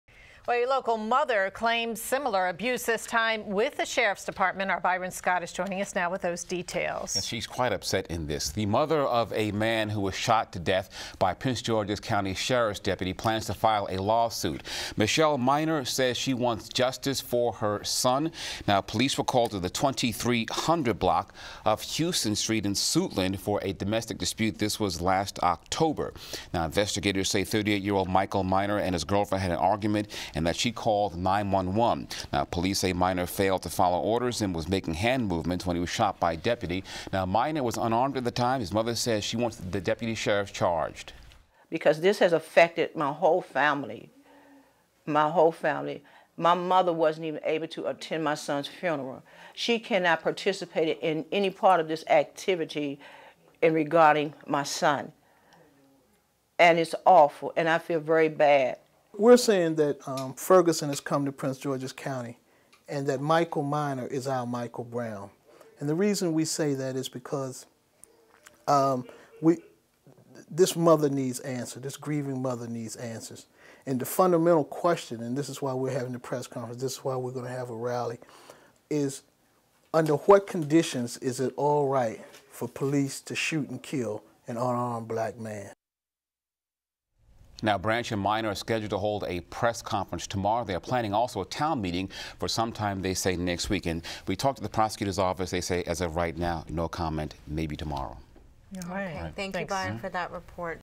Sheriff Shooting
CTV's Byron Scott does a sit-down interview with Michele Minor, the mother of Michael Minor, who was shot by a Prince George's County Deputy Sheriff in October 2014.